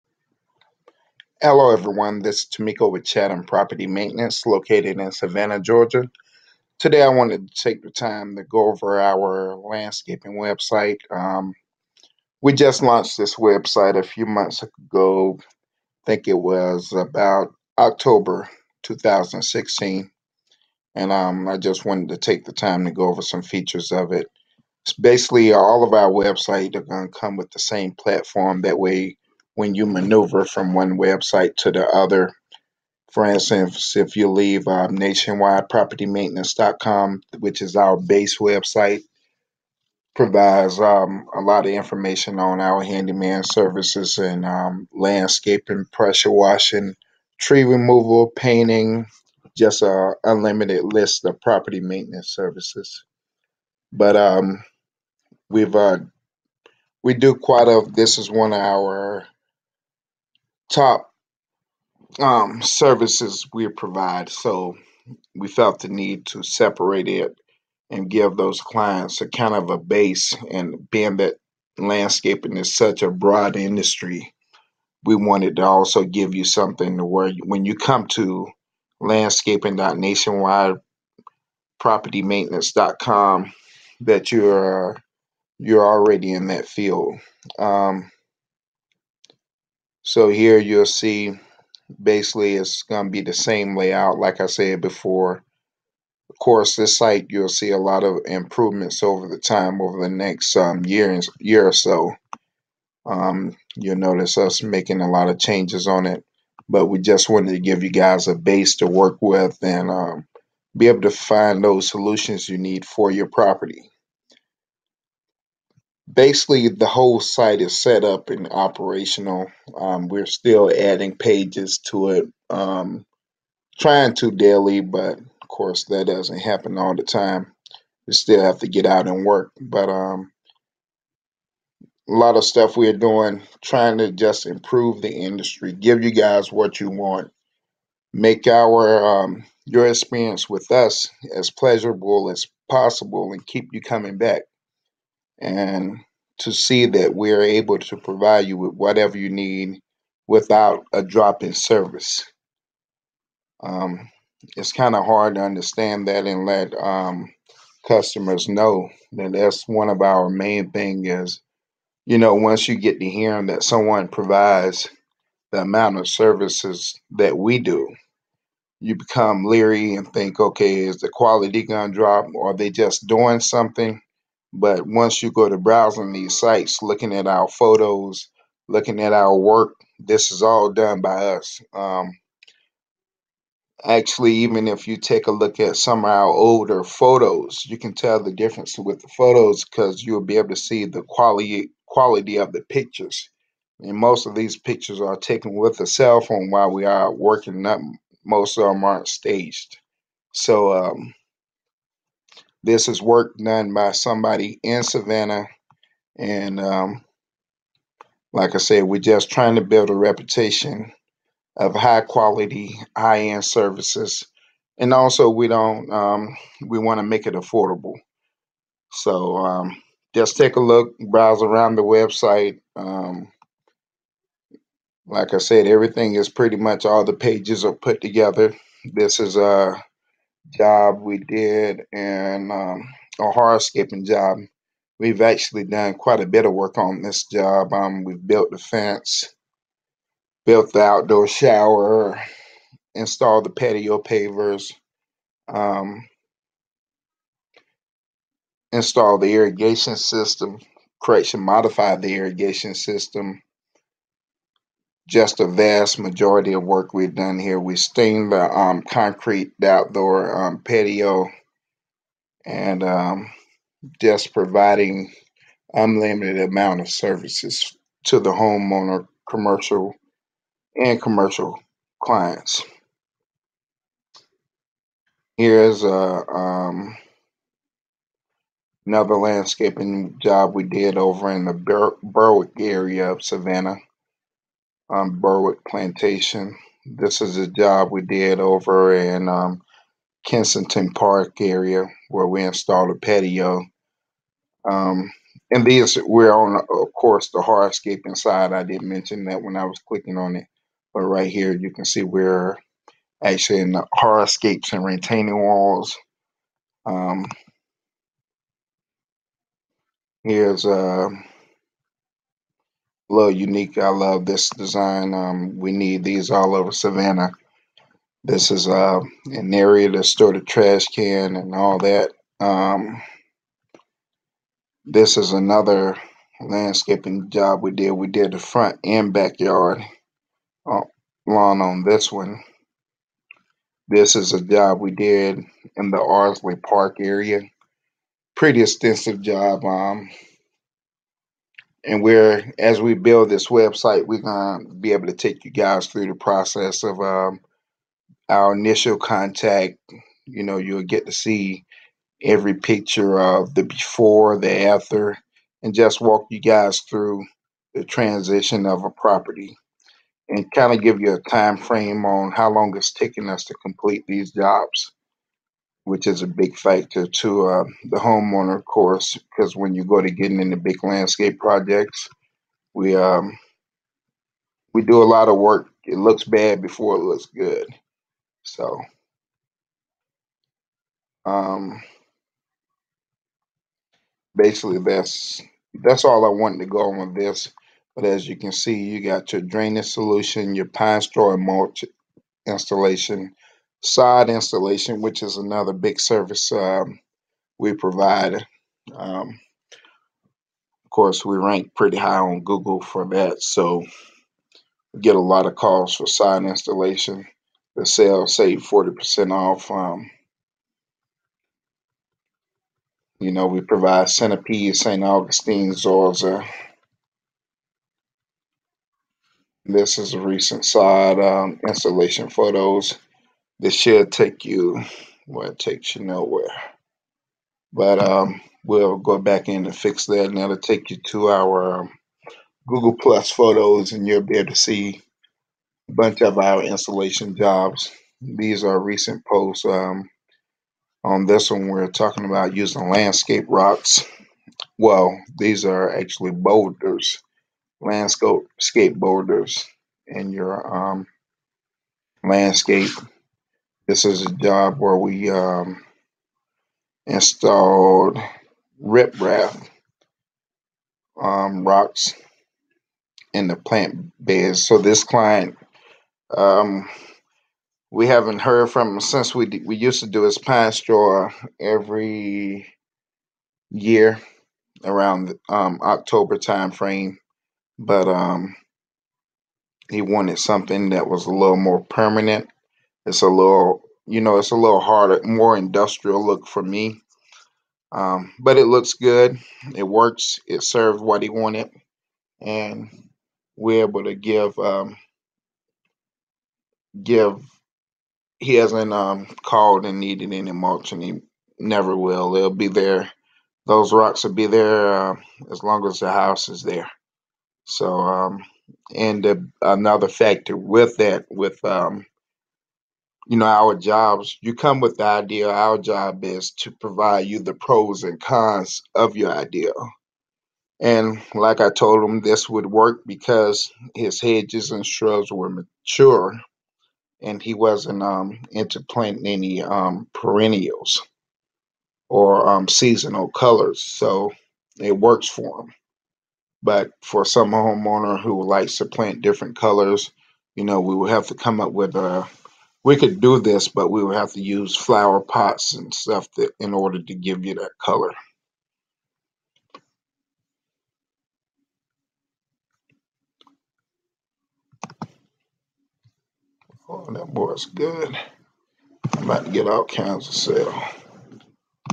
Basic overview of our Savannah Landscaping Website
Overview of the use of our landscaping website for landscaping services in Savannah, Pooler, Richmond Hill and Rincon